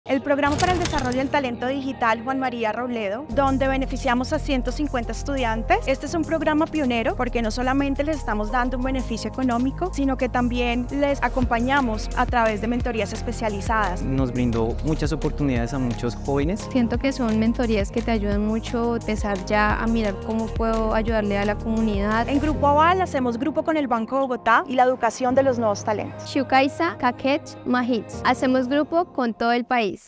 En Grupo Aval hacemos grupo con el Banco de Bogotá y quienes cuidan nuestra biodiversidad
Por eso hacemos grupo con Banco de Bogotá y un grupo de 150 jóvenes del programa "Juan María Robledo", pioneros en el desarrollo del talento digital en Colombia.